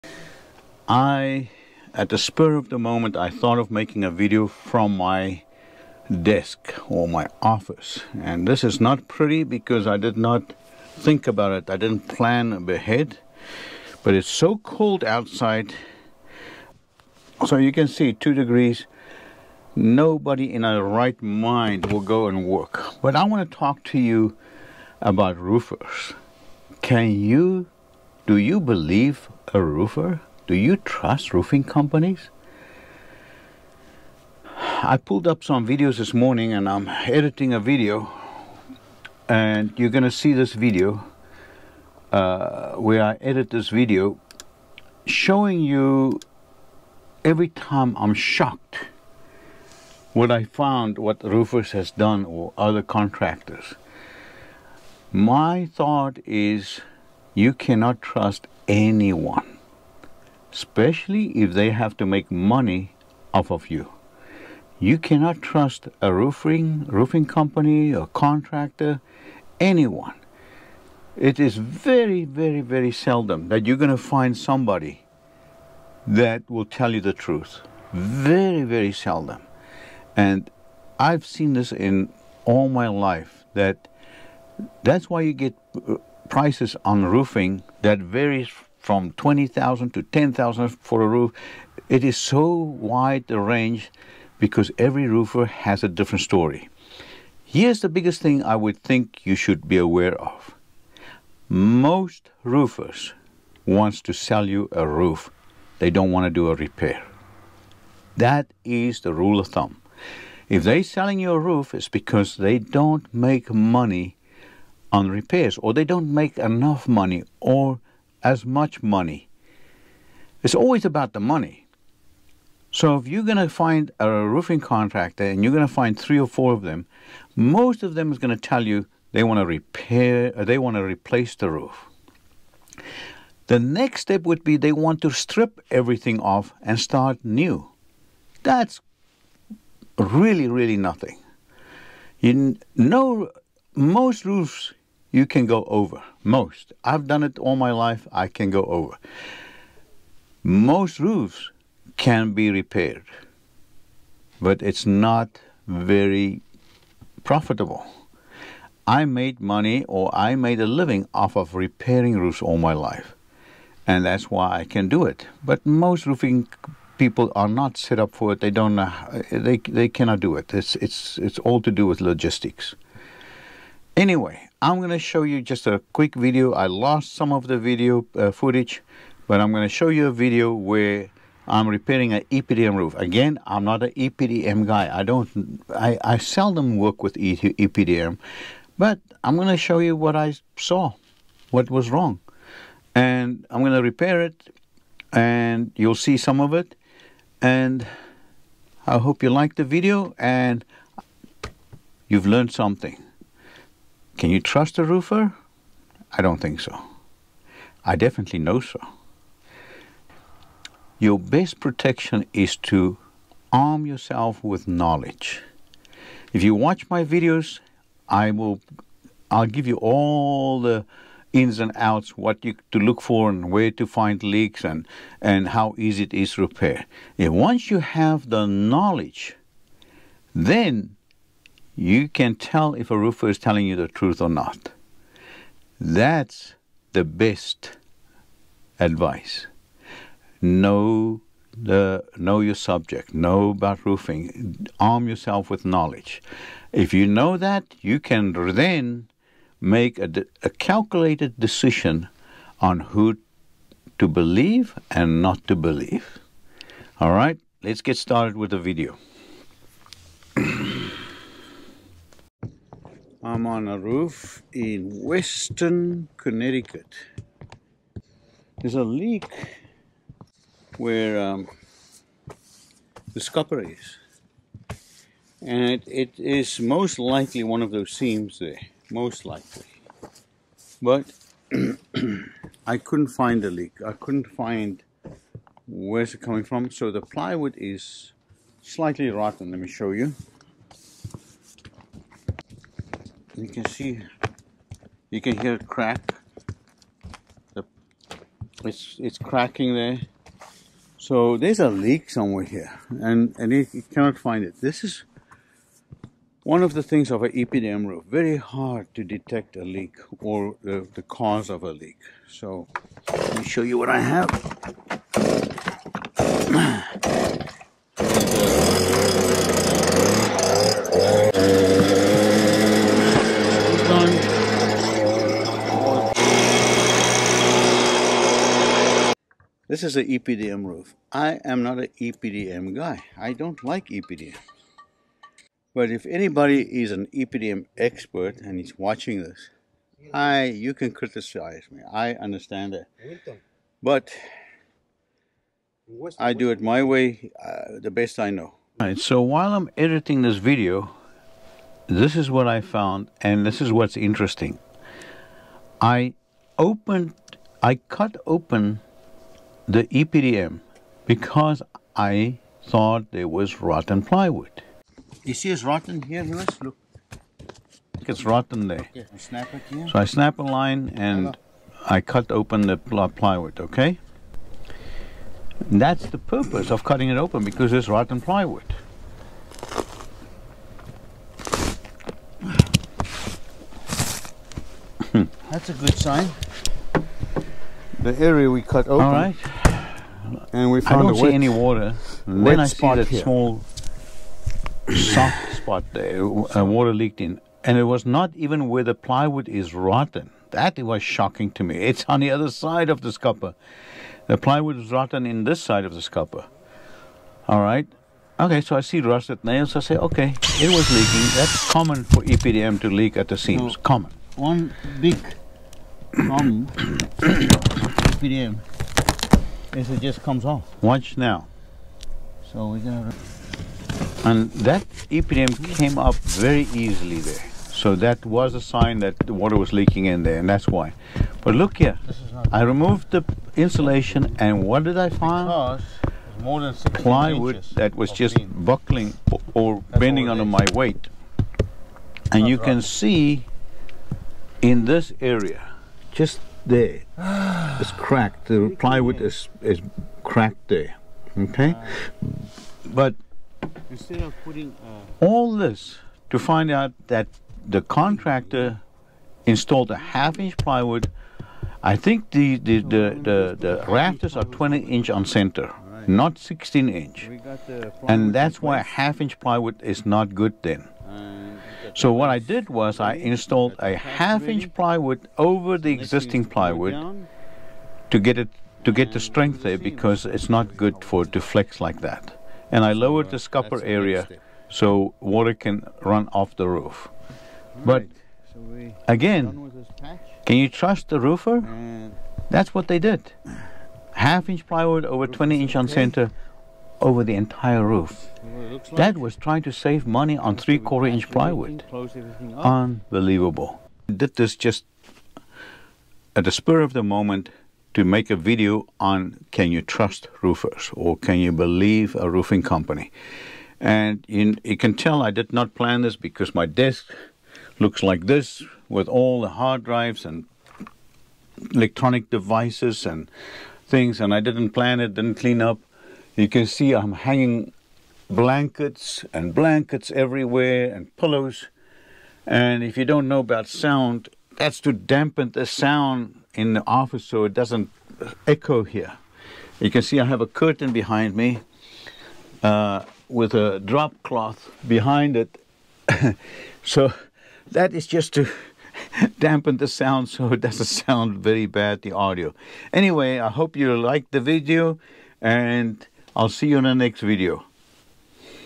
You are Crazy to believe a Roofer!! I don't even trust myself
Why would anyone believe a roofer or roofing company? It is insane to believe they have your interest in their heart.
All car salesmen, roofers, dentists, doctors have one thing in common, money. They will always try to get the easiest and most money for less work.
My wife just commented as I was writing this" now you show your true colors" She was a Dental assistant for 23 years, and she can attest the same for dentists.
So what can you do about it? watch the video to see my opinion and thiughts